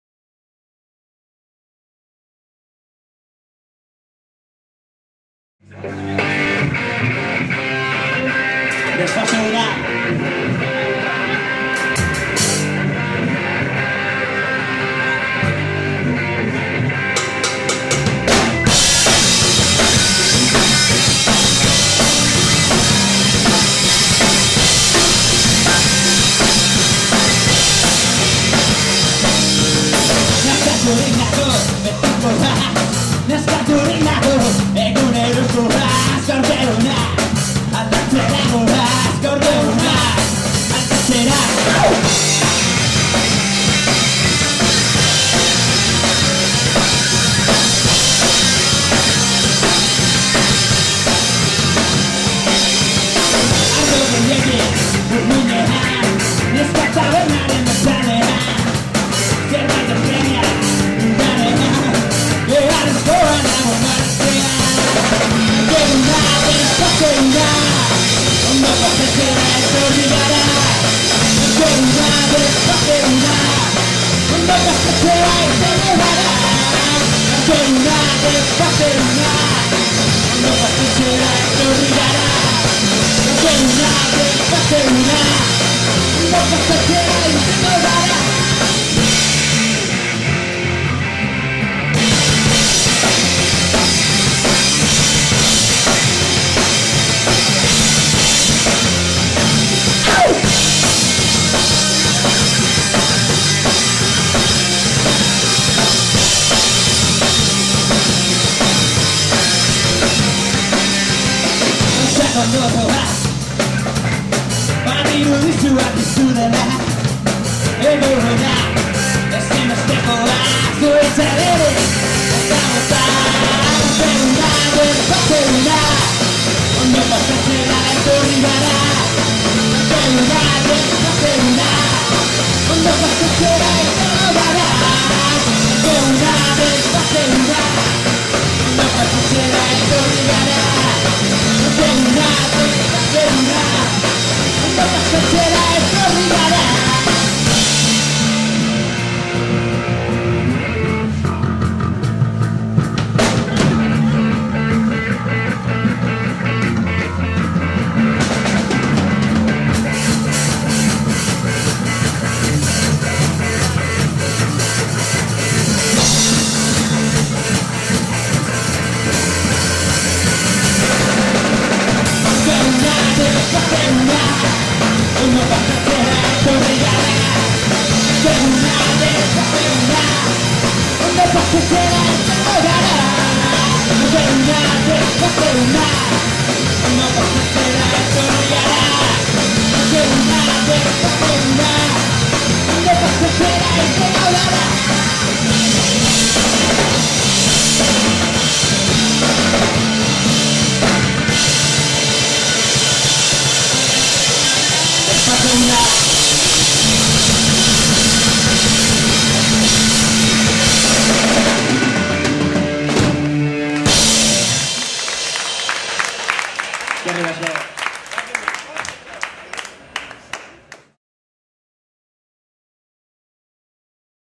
USTELAK #3 @ Crazy Horse 05/10/2011
Último tema de Ustelak que os mostramos dentro del circuito de bandas bilbaínas Barruti Rock.

Si quieres participar en este circuito, apúntate en la musik lista de grupos: